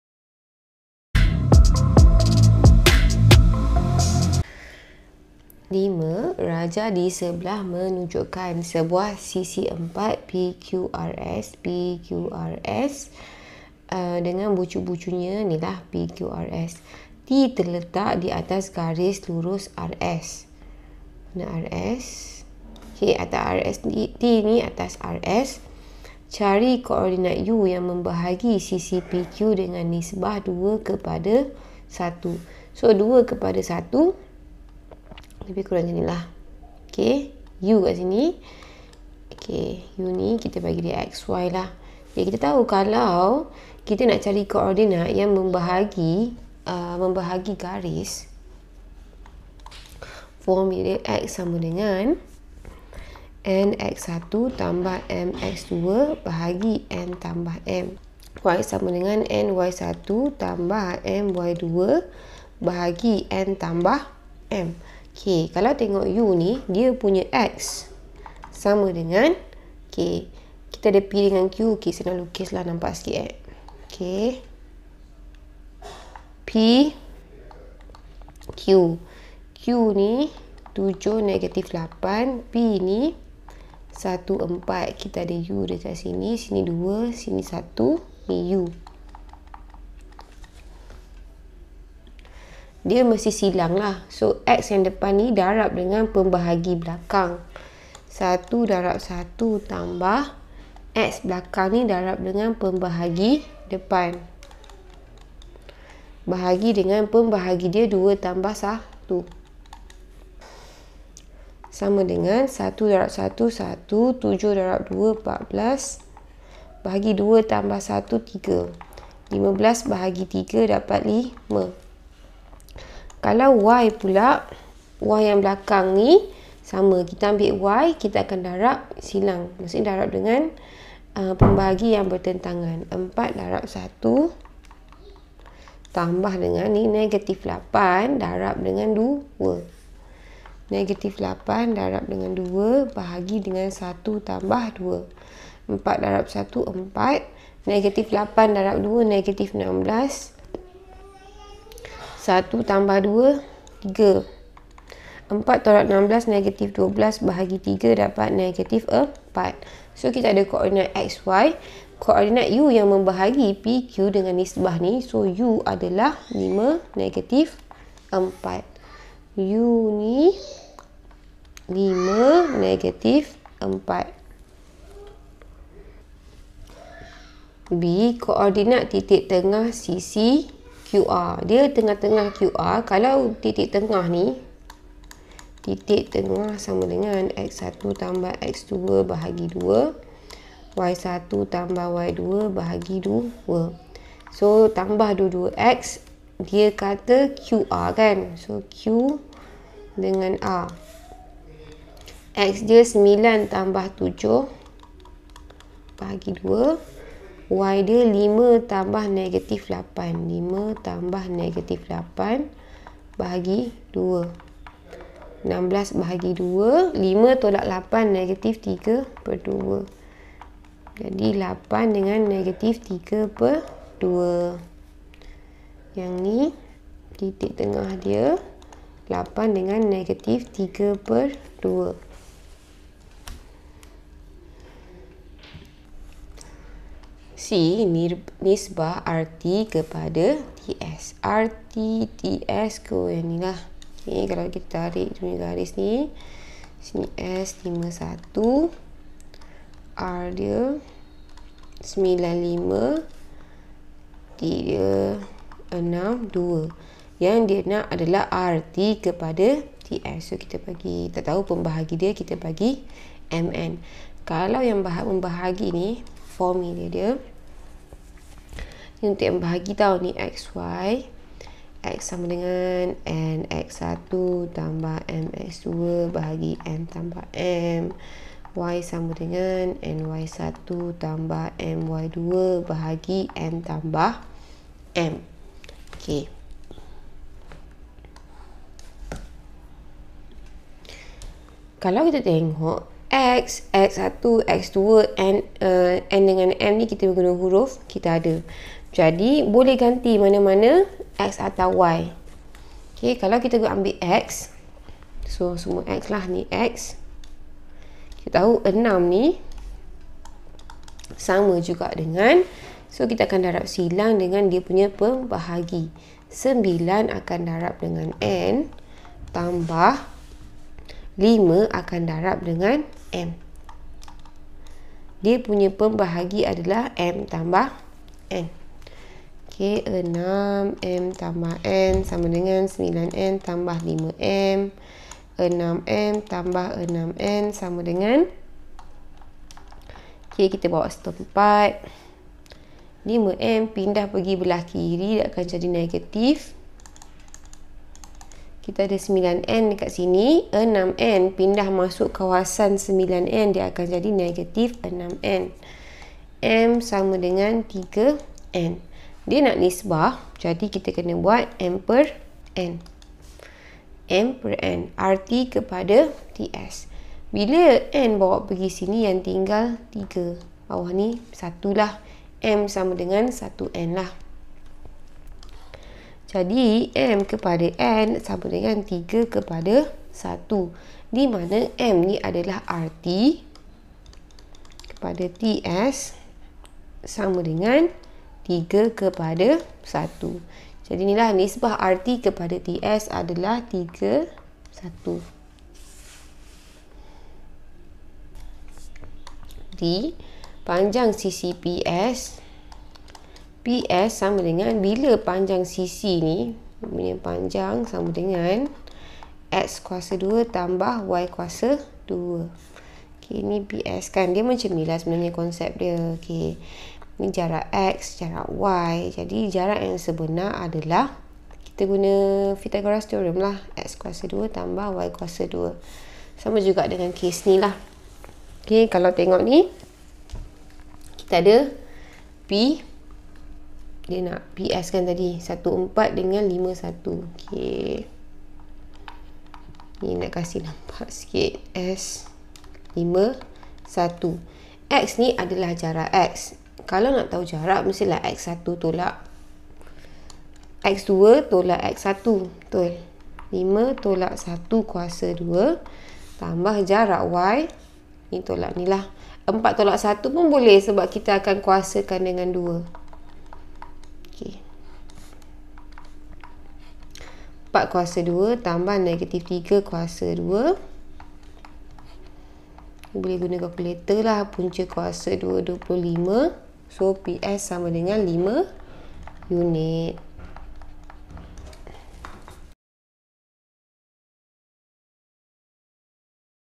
Latihan Intensif 7.1 No 5 | Bab 7 Geometri koordinat | 7.1 Pembahagi tembereng garis | Add Maths F4
Latihan Intensif 7.1 No 5 | Bab 7 Geometri koordinat | 7.1 Pembahagi tembereng garis | Add Maths F4 | Matematik Tambahan Tingkatan 4

5. Rajah di sebelah menunjukkan sebuah sisi empat PQRS y dengan bucu-bucunya ialah P(1, 4), Q(7, –8), R(9, 5) dan
S(5, 1). Titik T(6, 2) terletak di atas garis lurus RS. Cari
(a) koordinat titik U yang membahagi sisi PQ dengan nisbah 2 : 1,
(b) koordinat titik tengah sisi QR,
(d) panjang sisi PS.